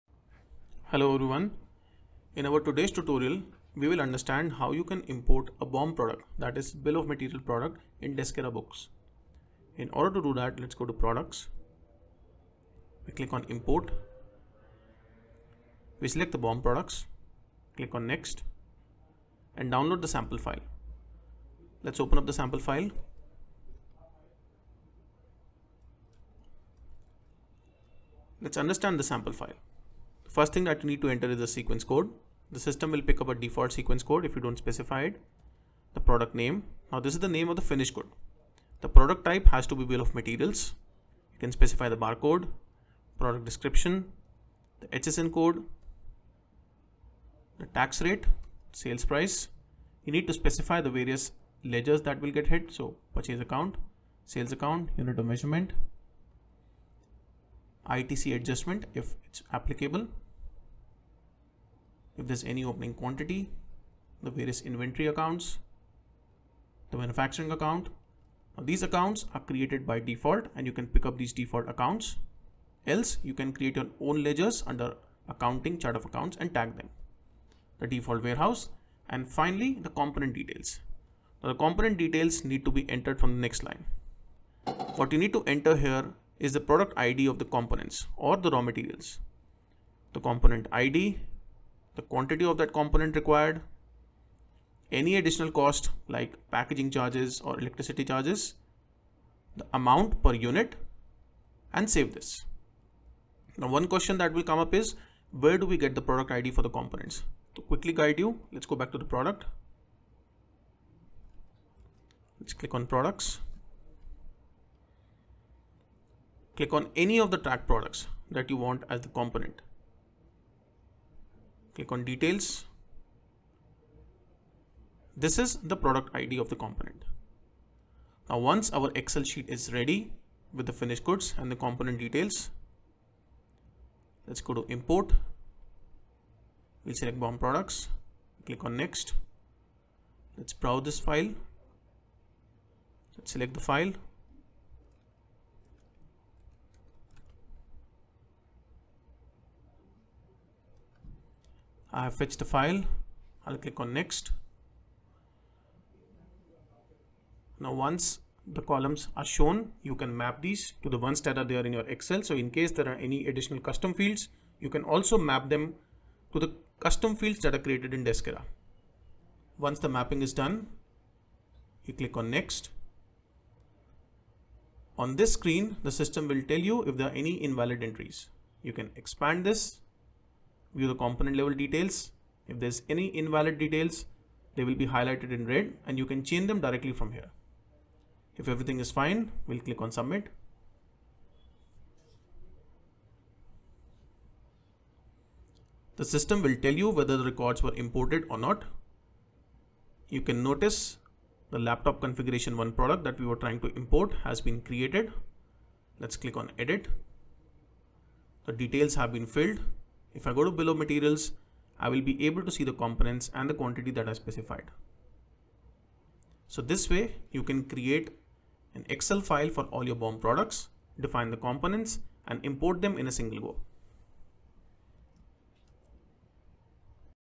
How to import BOM Products in Deskera Books
If you Assemble or Manufacture products and wish you import BOM (Bill of Materials) in Deskera Books, you can do so by simply populating details of the raw materials in the excel template and importing it in Deskera Books

Once you have created all Raw Materials in Deskera Books, creating the Finished Goods master only requires excel file import. This tutorial explains how you can quickly create the excel template and import it to set up all your BOM products